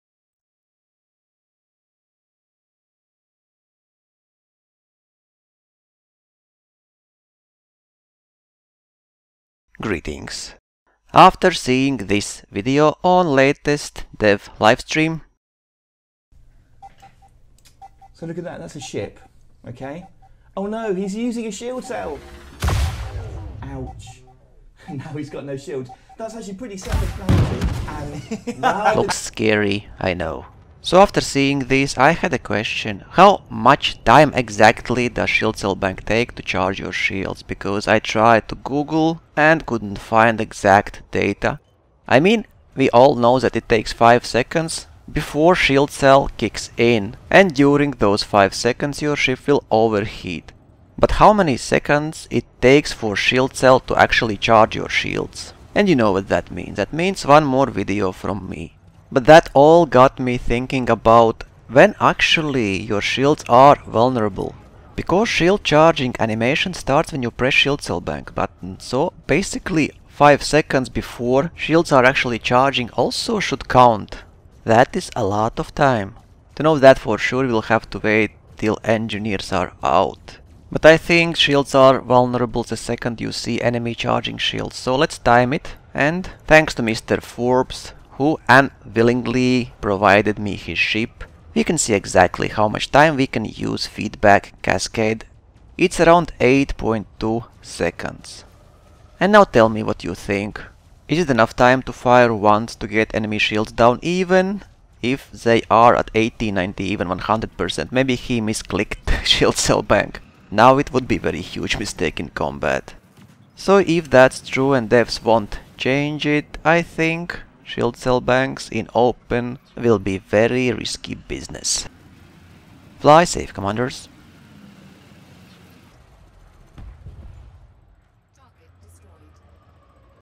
Feedback Cascade Railgun vs Shield Cell Bank. How much time we actually have?
After seeing devastating and scary Feedback Cascade Railgun in action on dev live stream I was wondering how it looks in combat.
I mean how much time actually shield cell is active and as nothing came up after a quick web search I went into combat and timed how many seconds you can visually see enemy charging their shields.